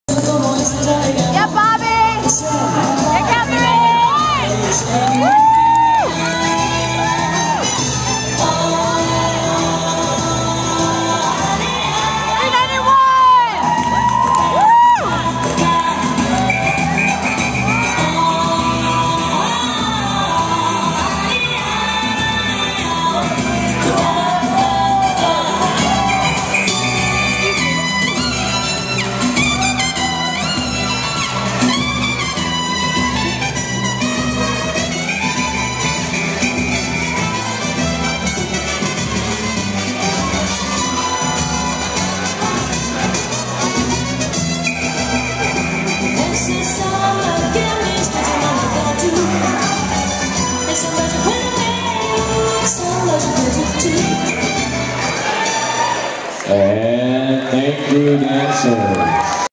Bronze Latin - Samba 1D - DCDI 2010 (Bobby and Katherine)
Samba First Round of the 2010 DC Dancesport Inferno (DCDI) Ballroom Dance Competition Bronze Amateur Latin event.

DCDI 2010 Bronze Samba
Bobby and Katherine DCDI 2010